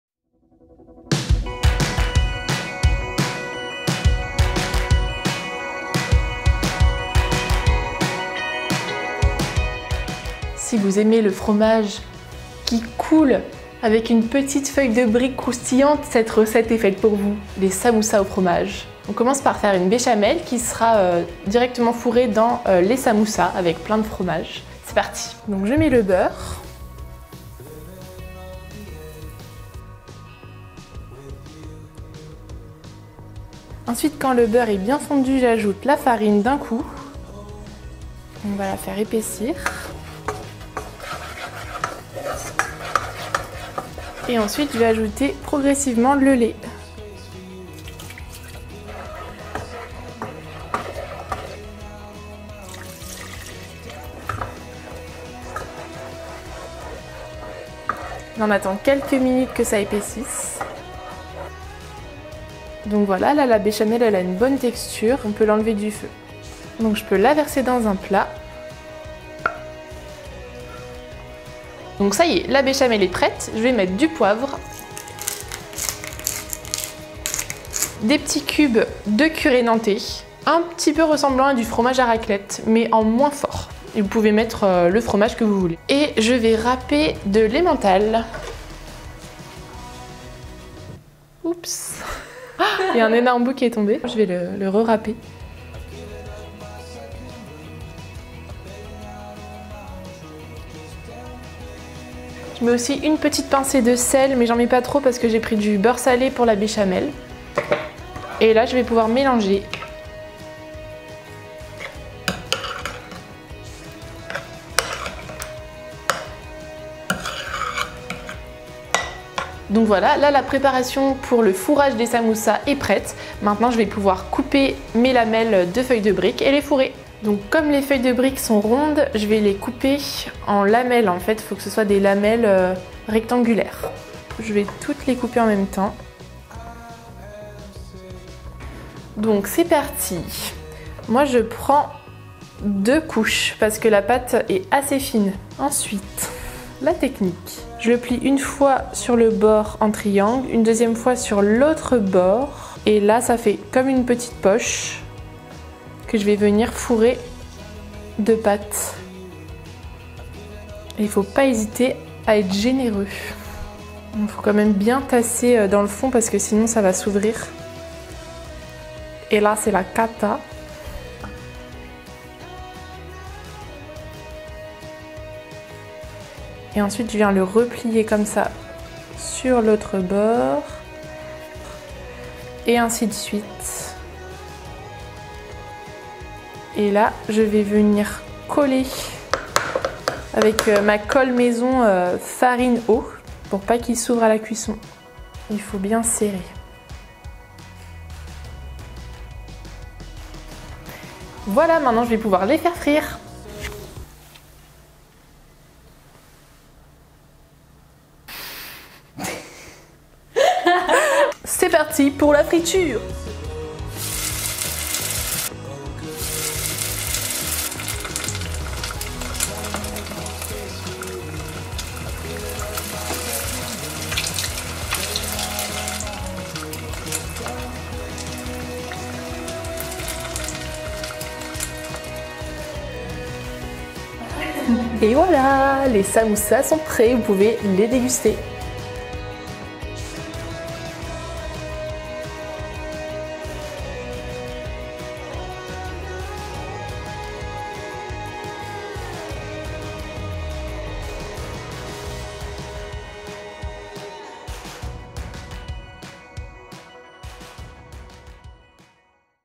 RECETTE DE SAMOUSSAS AU FROMAGE
Aujourd'hui on vous a concocté de délicieux samoussas au fromage, croustillant à l'extérieur et coulant à l'intérieur !
(Cette recette a été filmée avant la période du confinement)

LES INGREDIENTS 👇🏻

Pour la béchamel :

- 40 g de beurre
- 4 cuillères à soupe de farine
- 500 ml de lait
- Sel, poivre

- 2 paquets de feuilles de brick
- 1 curé nantais
- 1 gros morceau d'emmental (à raper et doser selon votre convenance)
- 500 ml d'huile pour la friture

 On espère que cette recette vous a plu ! A bientôt 👩🏻‍🍳🙋🏻